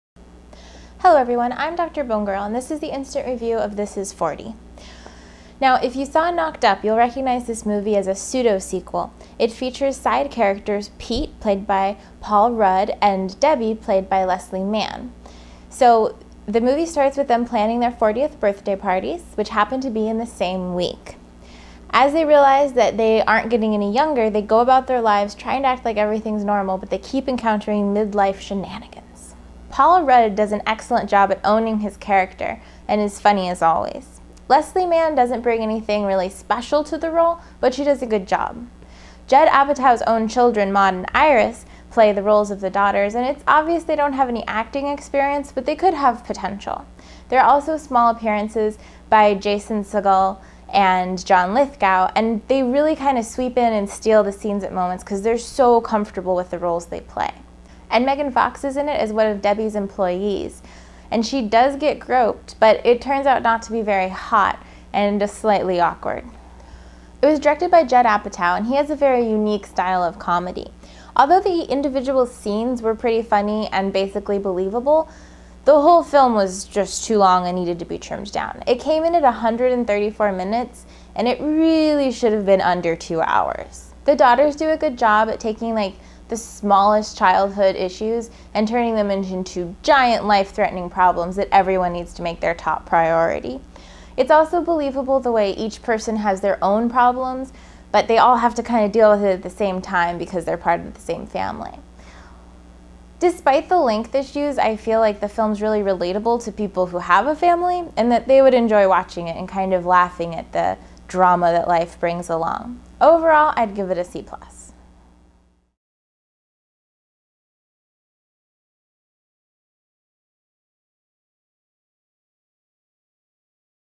Instant Review of This is 40 (minutes too long)
Review of This is 40. A pretty good movie with believable situations, but a very long runtime. Paul Rudd and Jason Segel are funny but the same characters as always. We gave it a C+